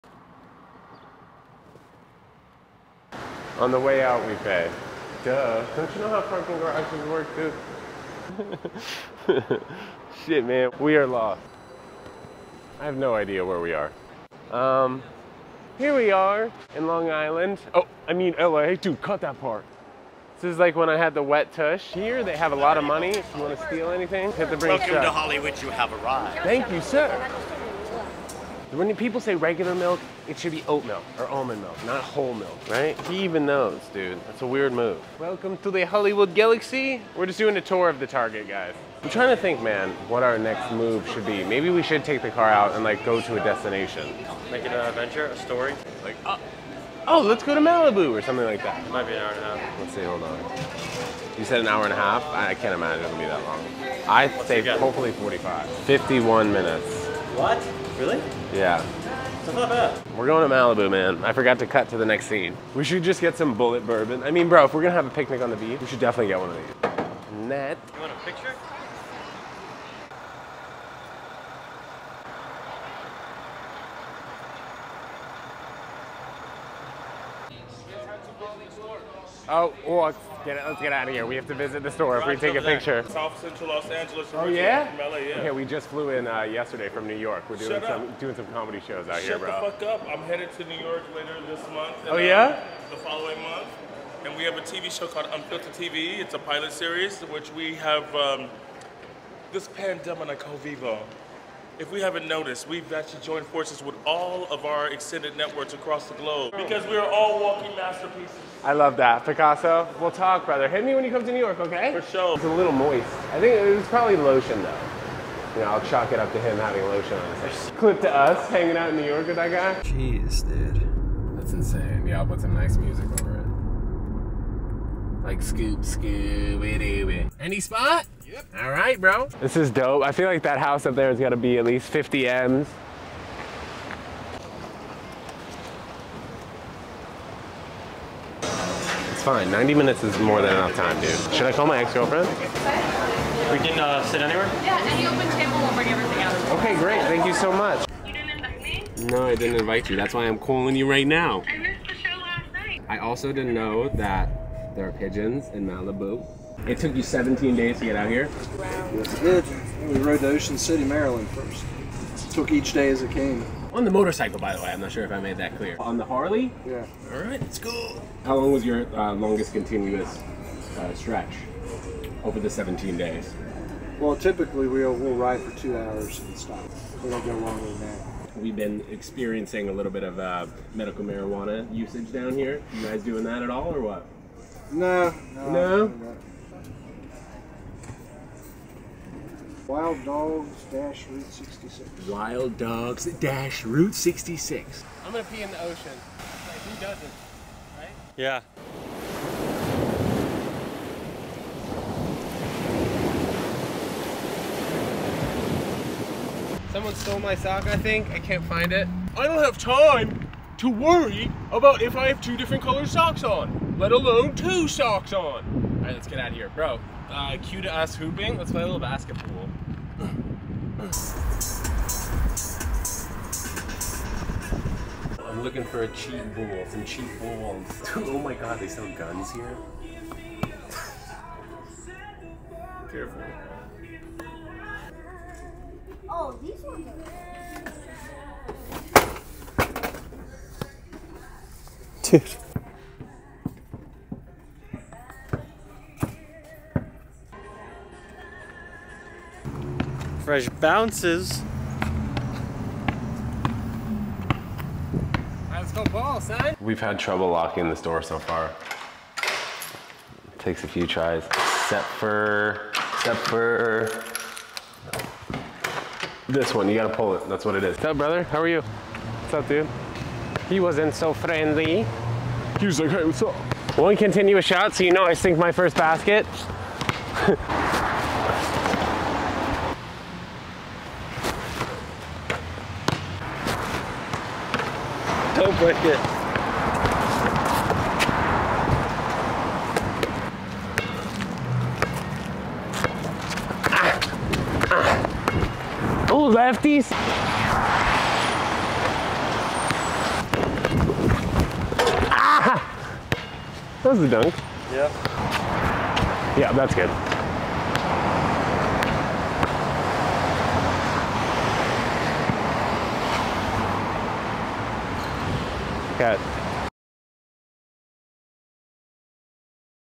BALLIN IN MALIBU!
Ted ventures to Malibu From HOLLYWOOD- and balls along the way :)